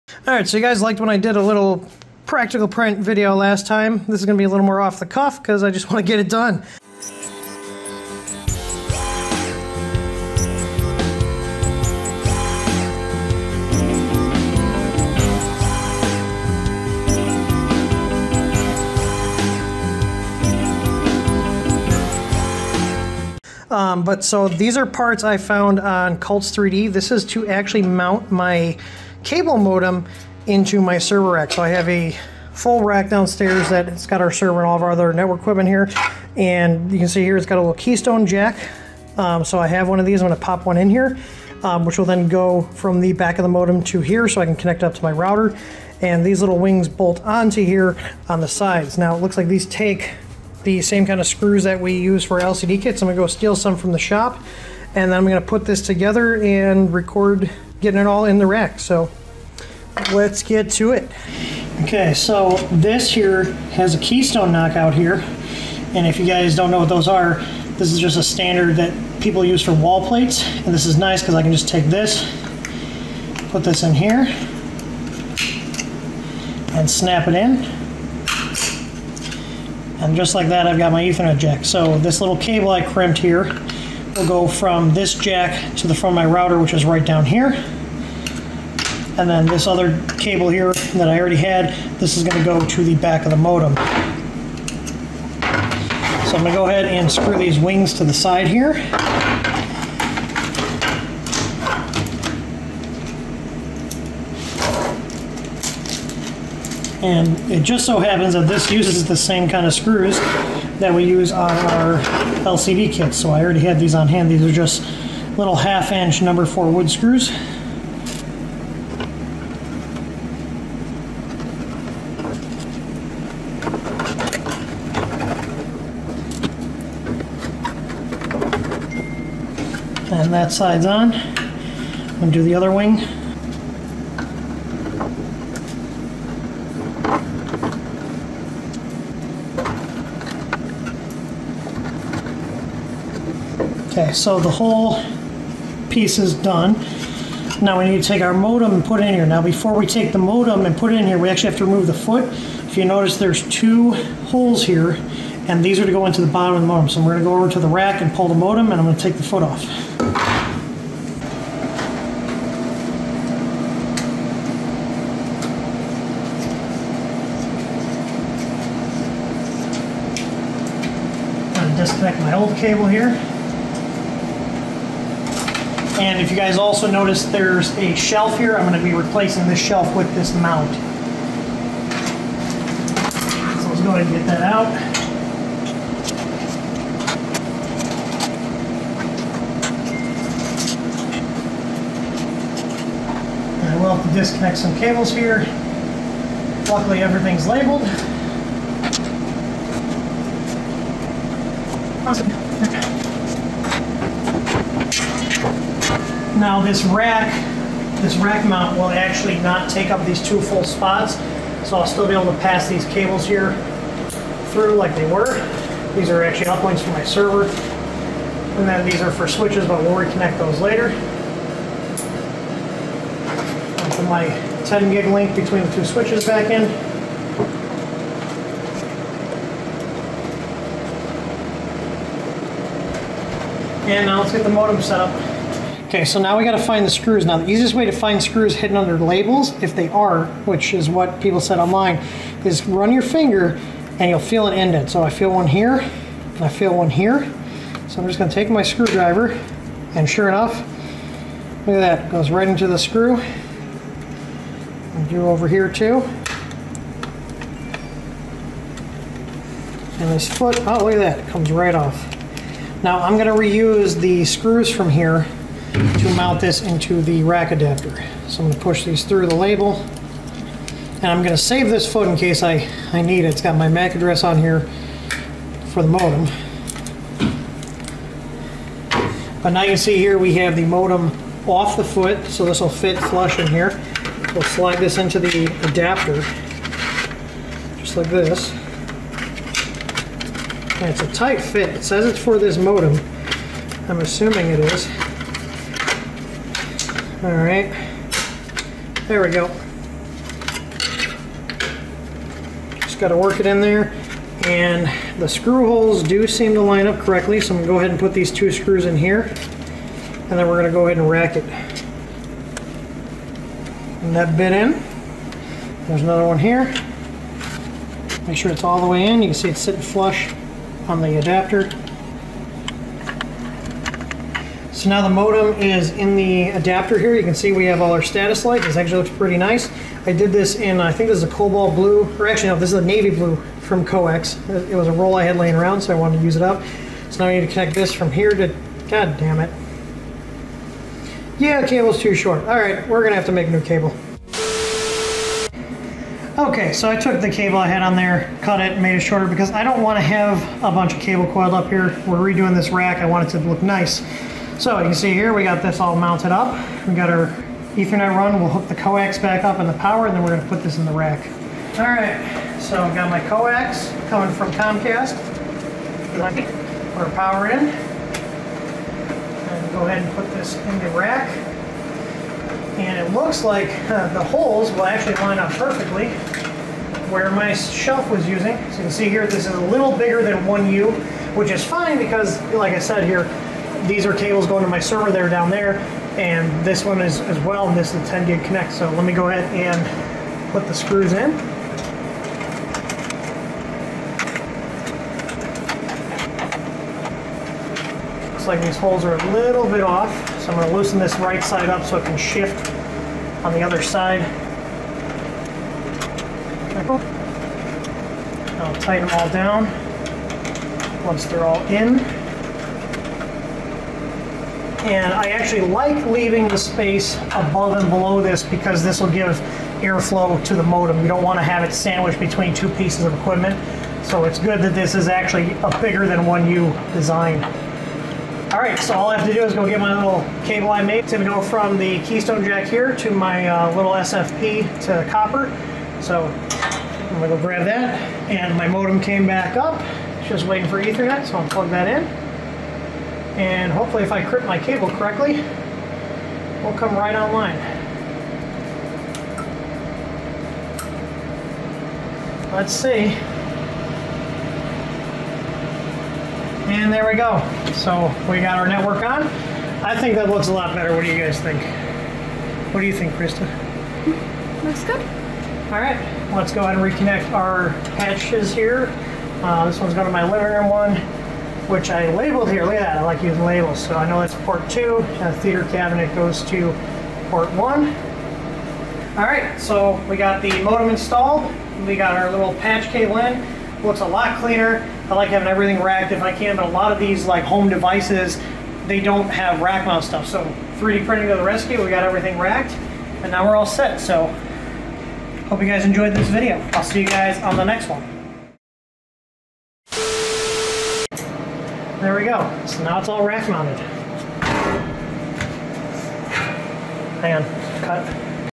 Practical Prints - Rack mounting my Motorola MB8611 Cable Modem
STL - This is the file I said cults3d in the video on accident